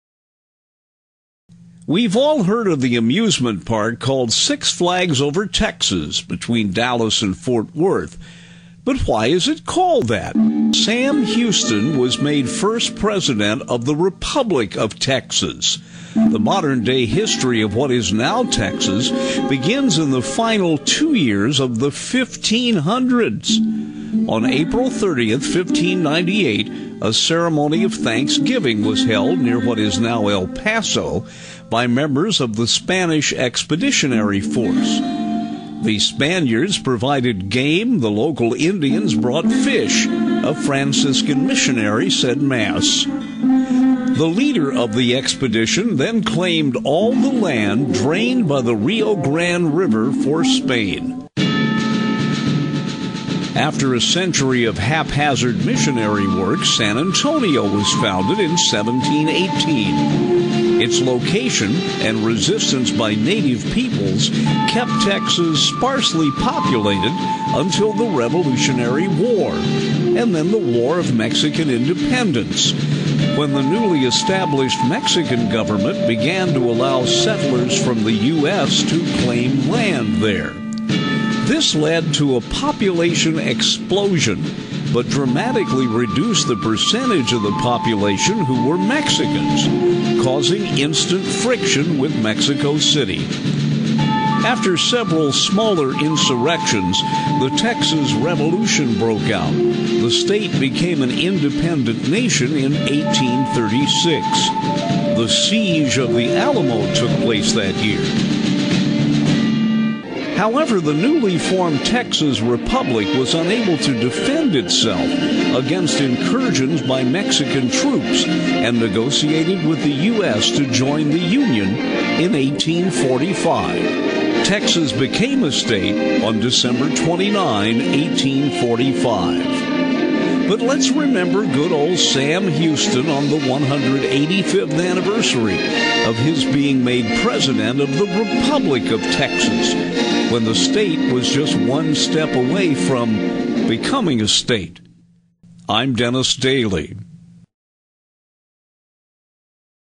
DENNIS and OCTOBER 22 -- An area under many flags for a long time ...
DENNIS DAILY looks at a major event in history. An American state has a grand heritage ...